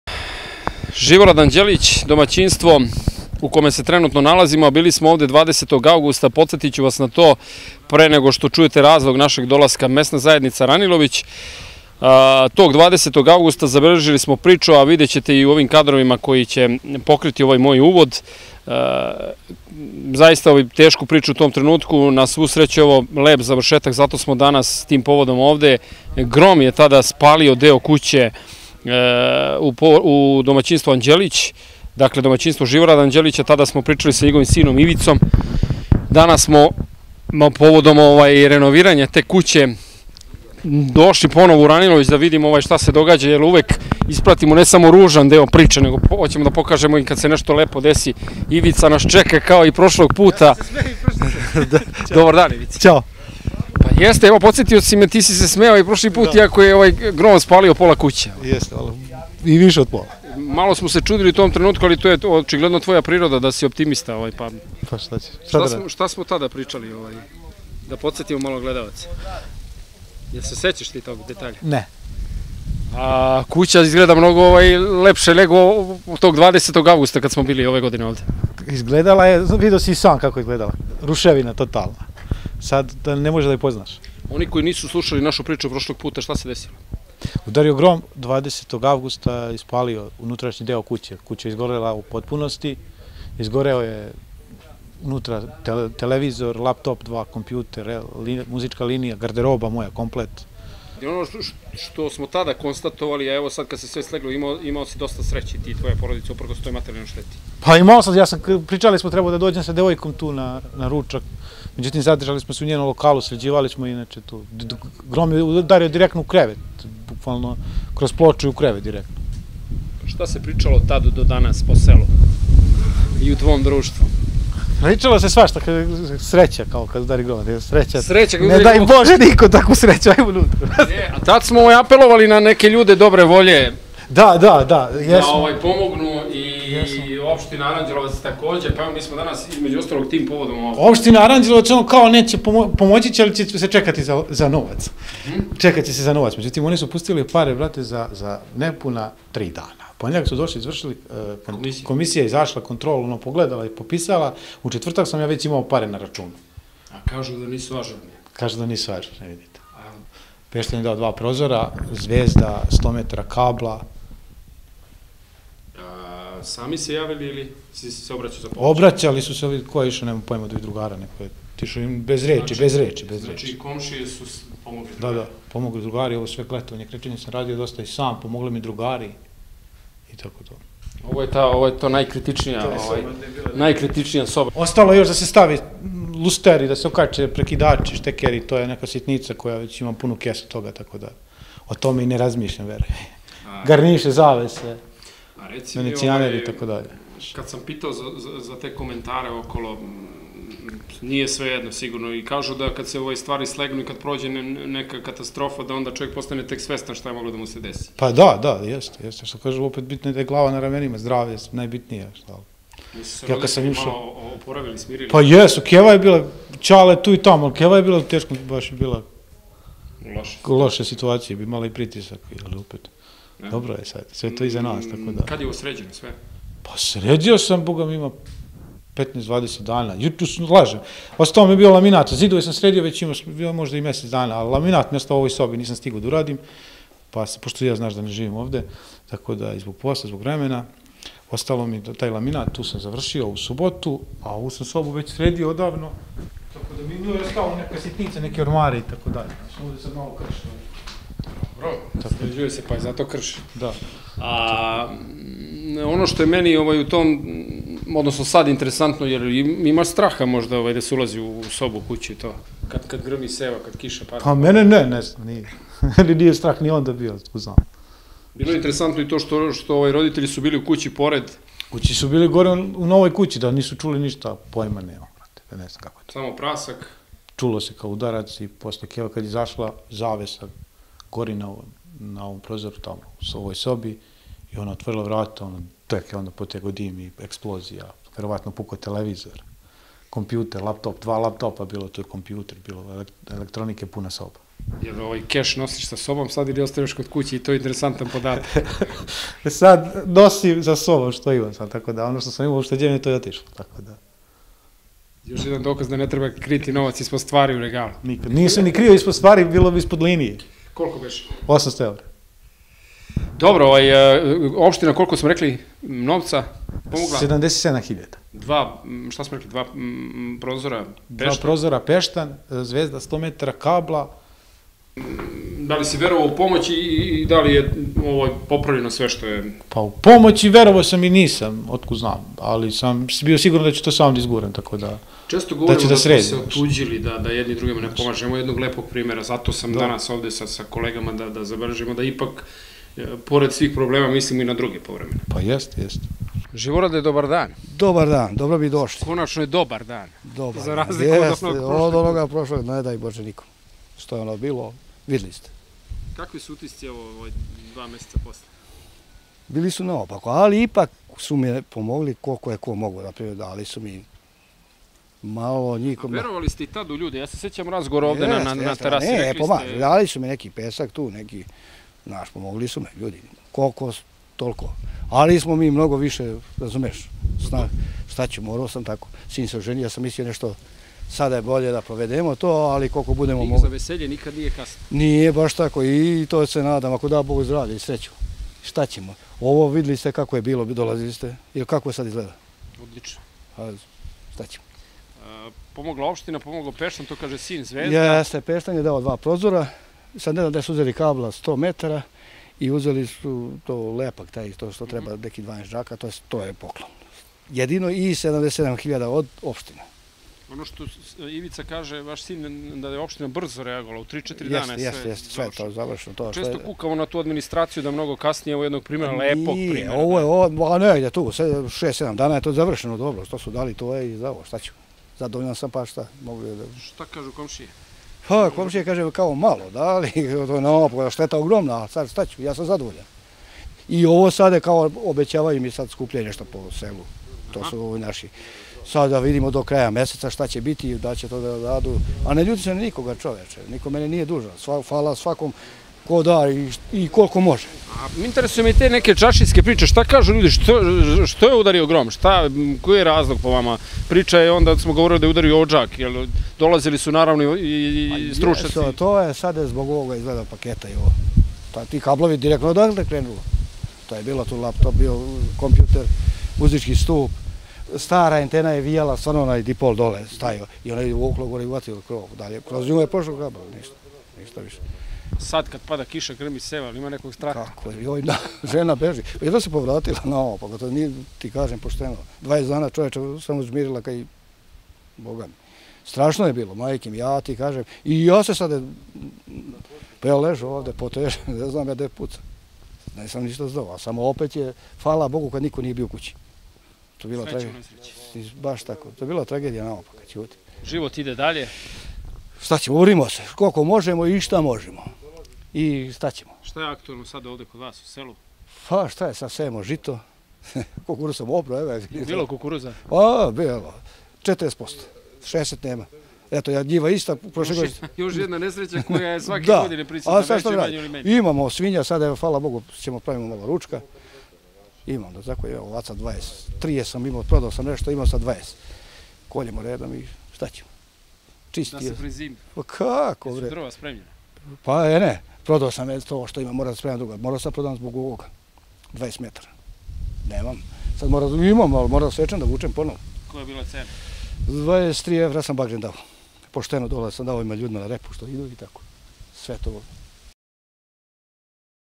RANILOVIĆ-PORODICA ŽIVORADA ANĐELIĆA-OBNOVA KUĆE
Danas (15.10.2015.) je novinarska ekipa RTV "Sunce" iz Aranđelovca obišla domaćinstvo porodice Anđelić u MZ Ranilović čije je domaćinstvo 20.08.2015.godine pogodio grom i deo kuće u potpunosti spalio.
Dva meseca kasnije , zahvaljujući ličnim sredstvima i sredstvima Opštine Aranđelovac , kao i pomoći kompanije Peštan , preduzeća Zvezda iz Aranđelovca i Elektrodistribuciji iz Aranđelovca kuća je gotovo u potpunosti renovirana.
Pomogli su kaže Ivica Anđelić i njegov otac Živorad i pojedini rođaci,prijatelji i komšije...

Prilog RTV "Sunce" iz Aranđelovca